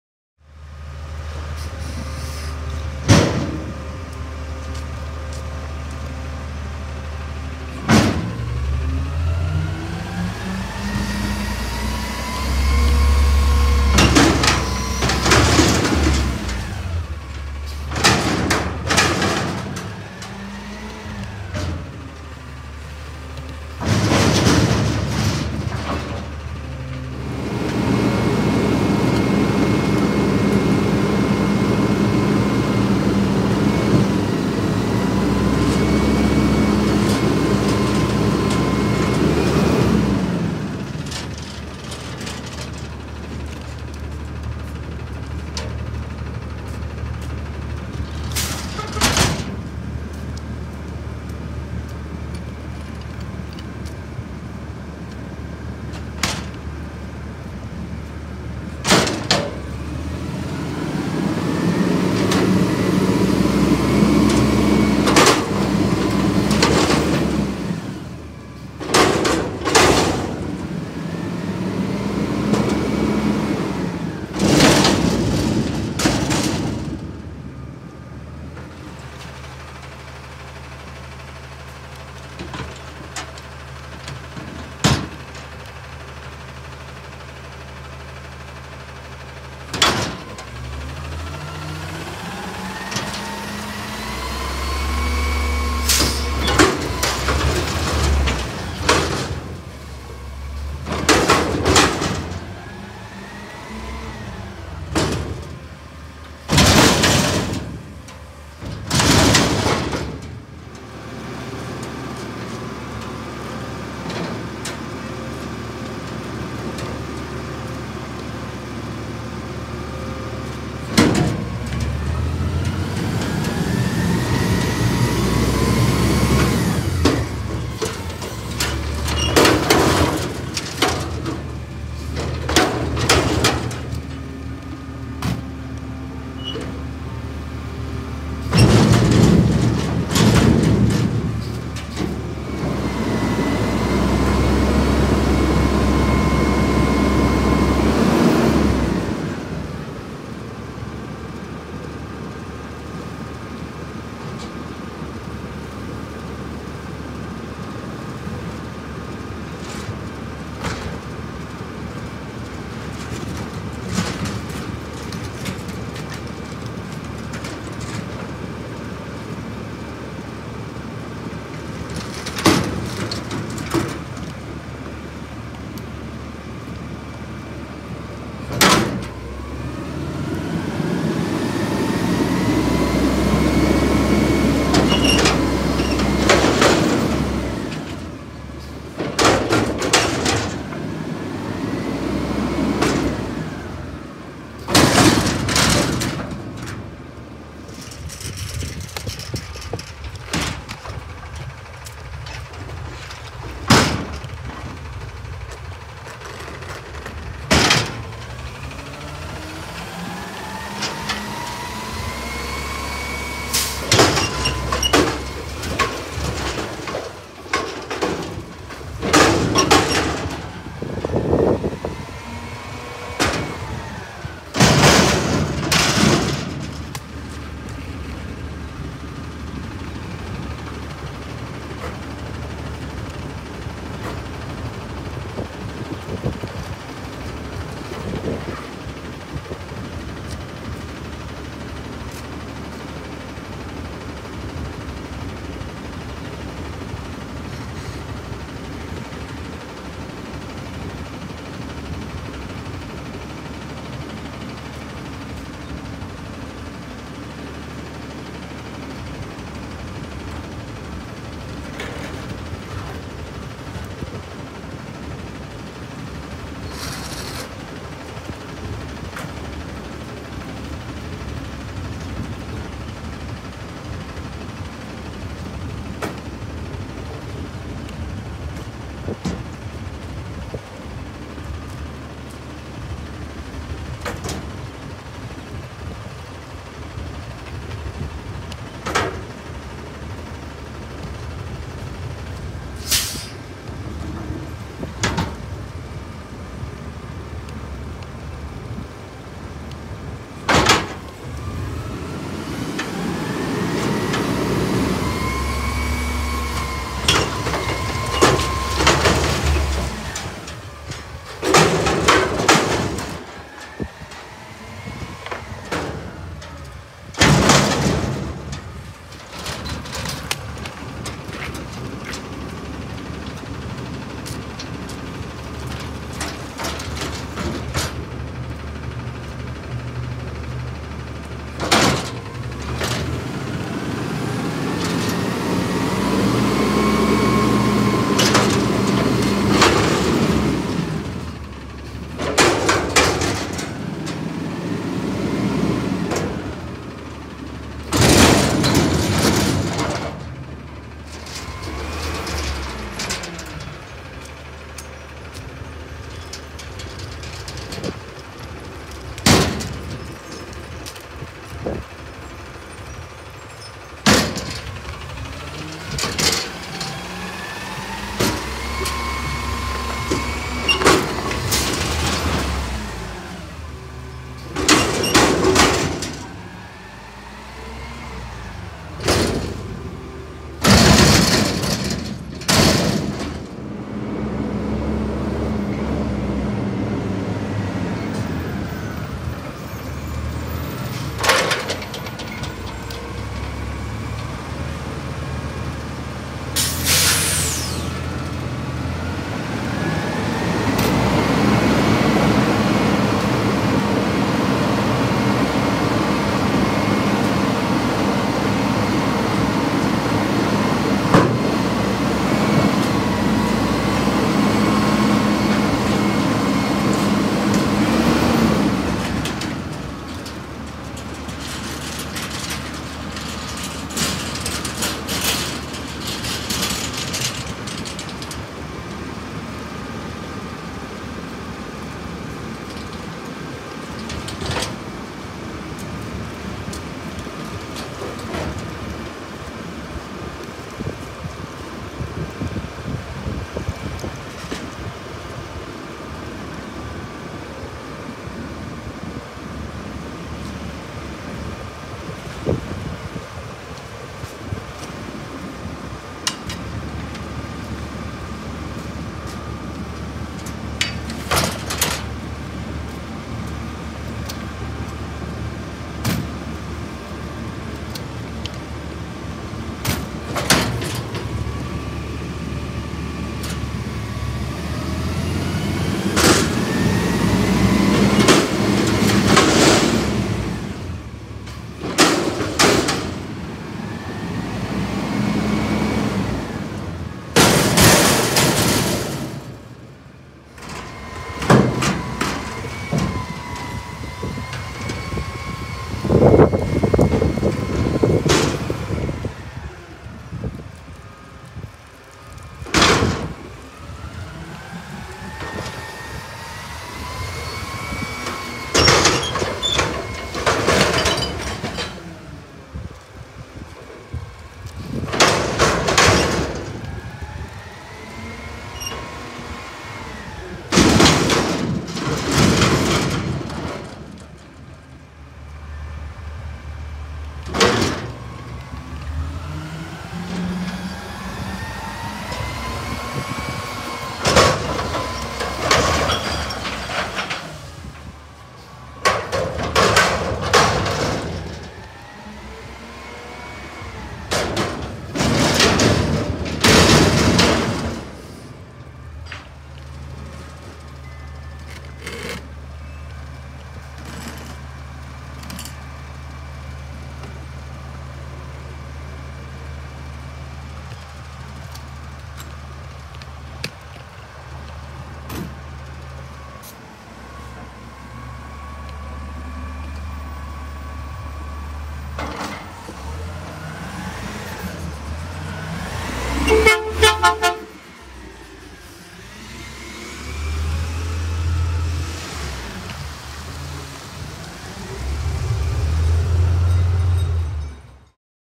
Mobile Home Park Dumpster Pickup by Athens Services
Starting the week off right by watching dumpster pickup.

We went to Azusa and saw a pickup in a mobile home park.

Got to see some really full ones today.

I knew the driver - nice guy.

955 Athens Services Autocar ACX McNeilus on Trash Dumpsters